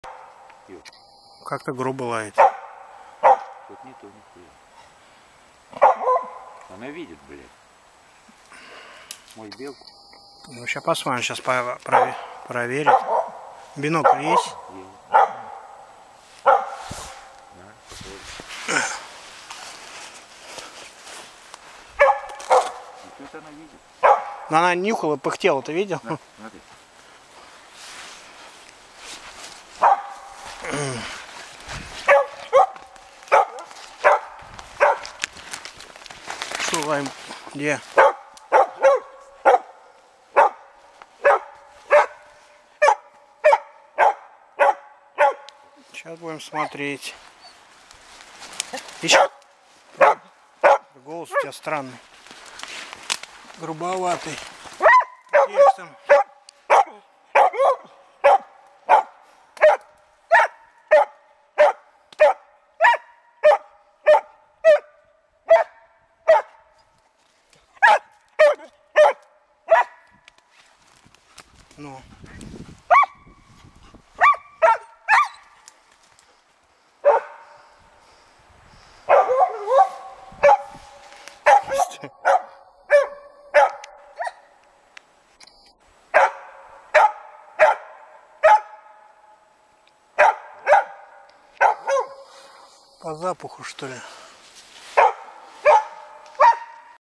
ЛАЙМА МНЕ ПОКАЗЫВАЕТ ВЕРХ!!!
Это классика лайки когда собака  работает по верху.А Лайма работает всё.И  низ и верх.Моя красота.И помощница.Ещё порадую вас  работой лаек есть время  до конца февраля.